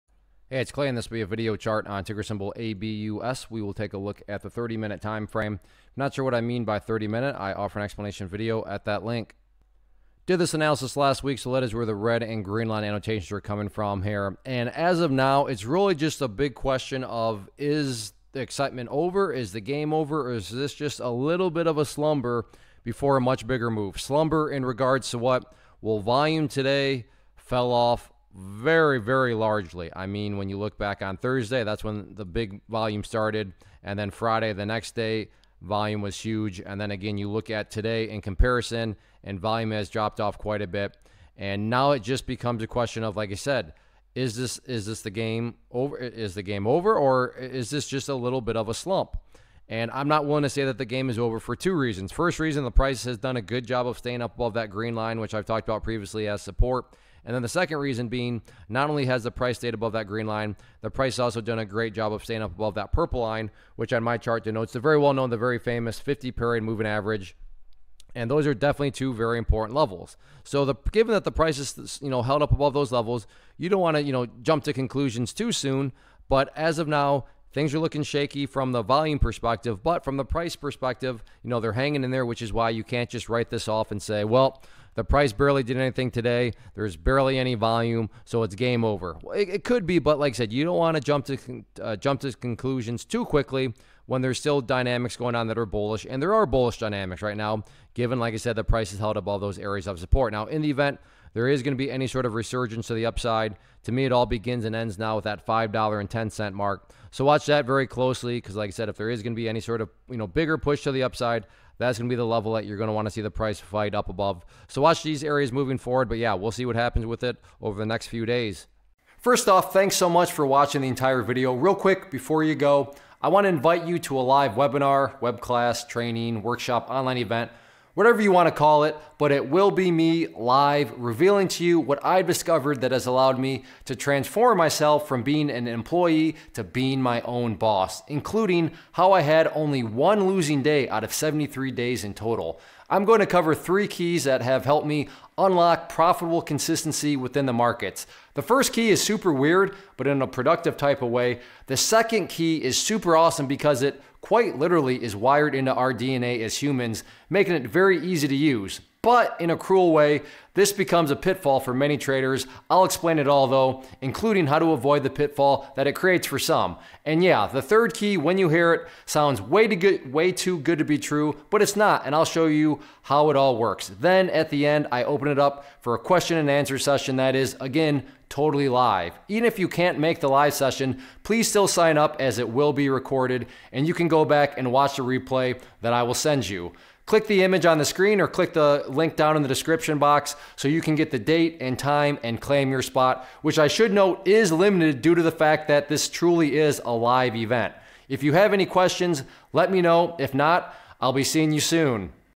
Arbutus Biopharma Corporation - ABUS Stock Chart Technical Analysis for 07-27-2020
Learn how to read stock charts and identify technical patterns as ClayTrader does a quick stock chart review on Arbutus Biopharma Corporation (ABUS).